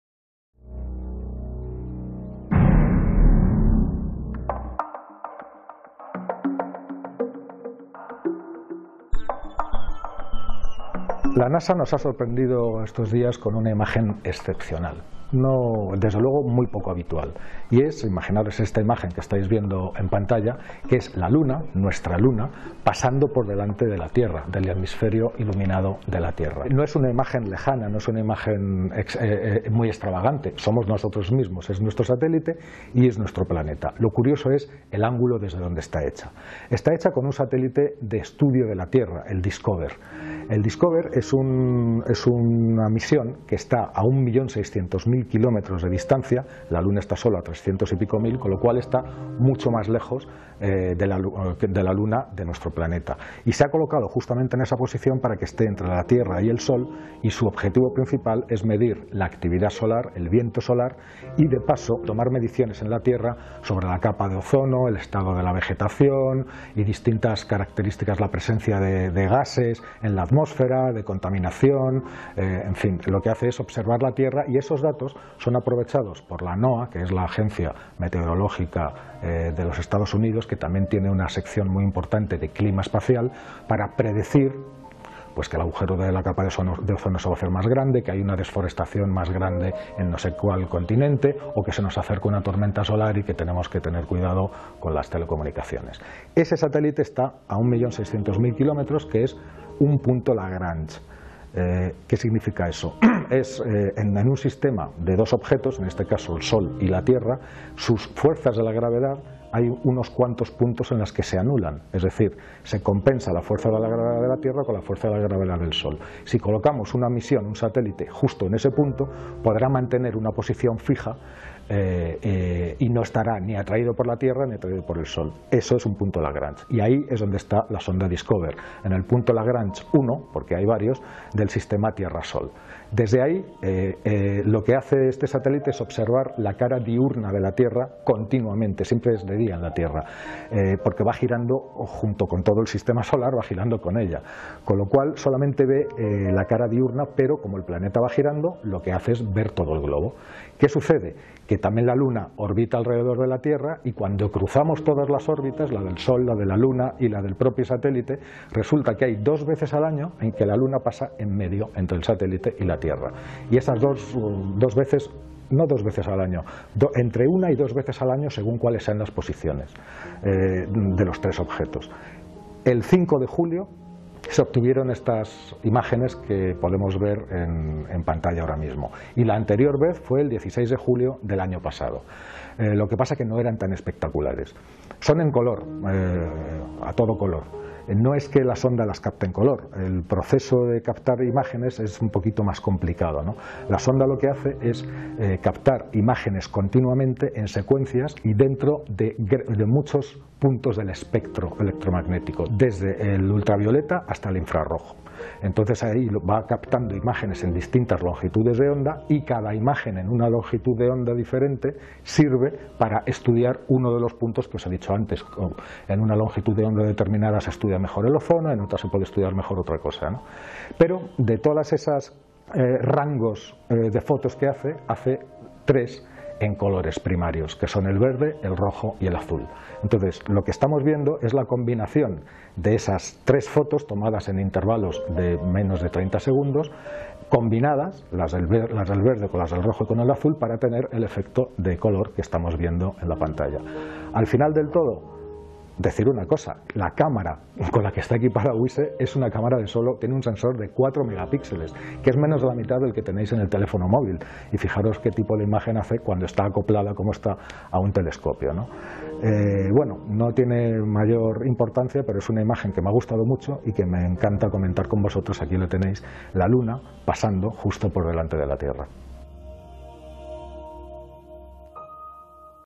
La Luna pasa por delante de la Tierra
Impresionante imagen de la Luna la que ha captado un satélite de la NASA, en la que puede verse cómo pasa por delante de la Tierra. La sonda DSCOCVR (Deep Space Climate Observatory), orbita muy lejos de la Tierra, a más de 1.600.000 km.